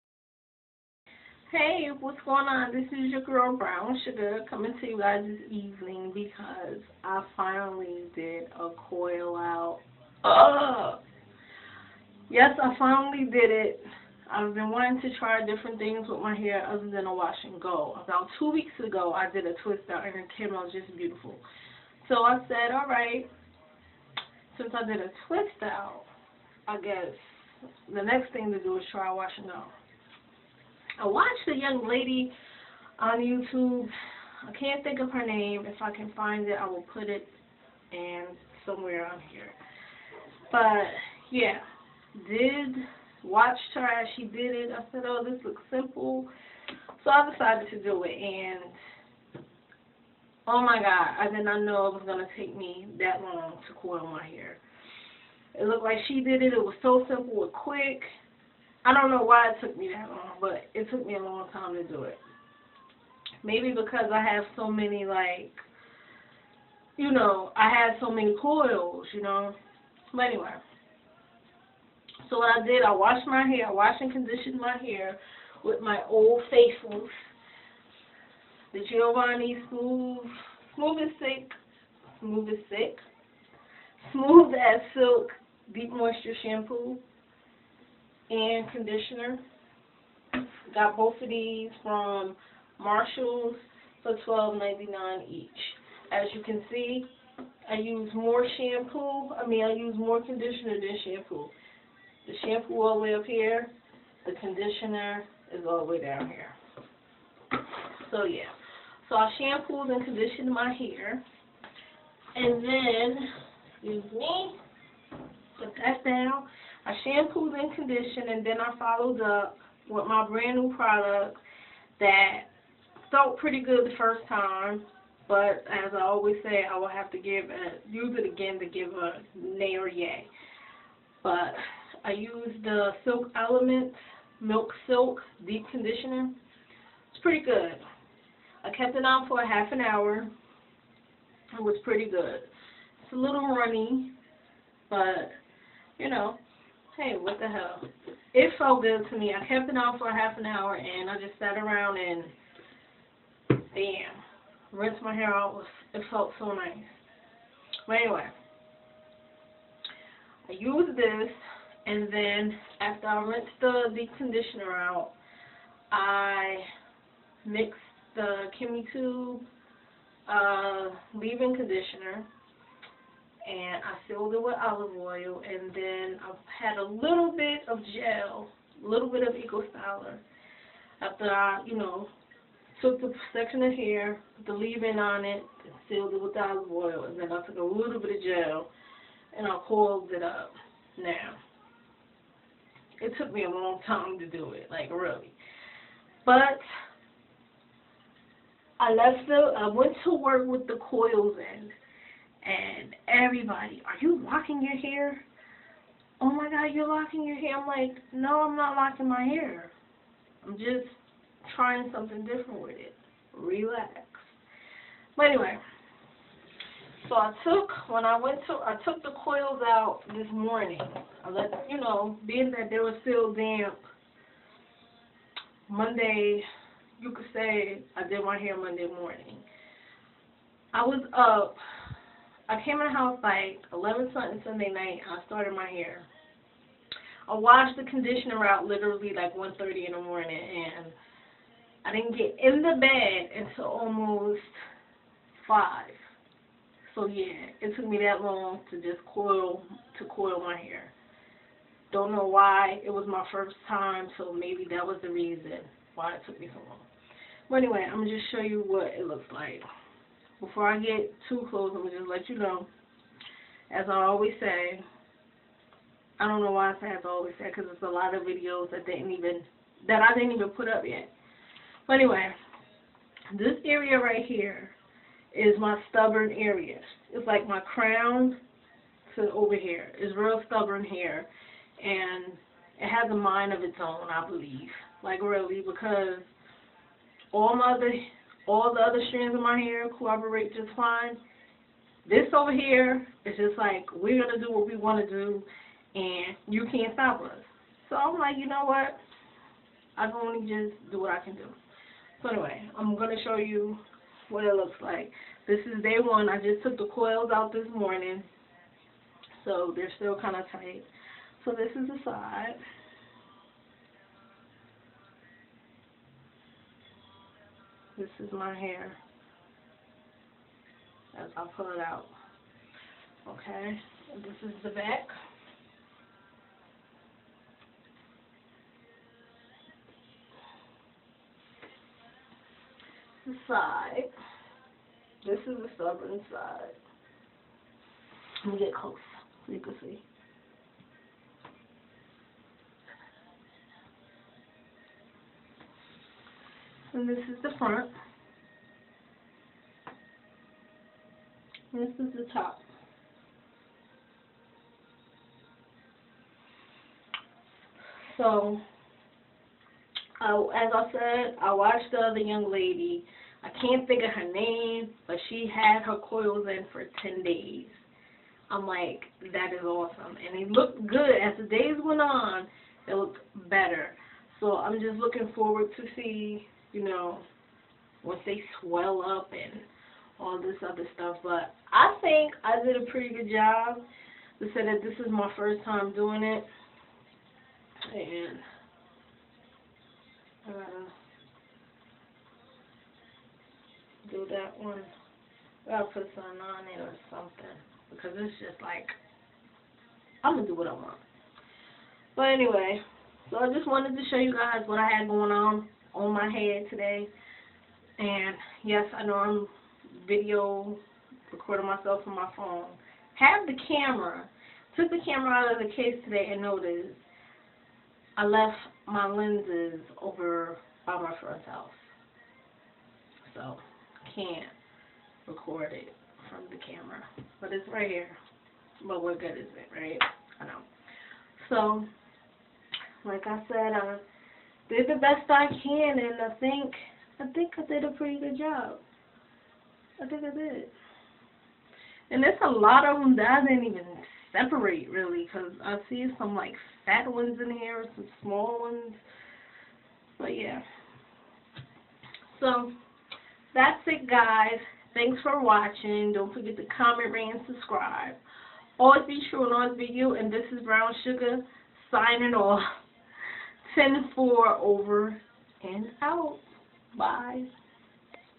Finger coil-out on my TWA: Styling your TWA
This Video was recorded February 2012 did finger coil out on my TWA and I love it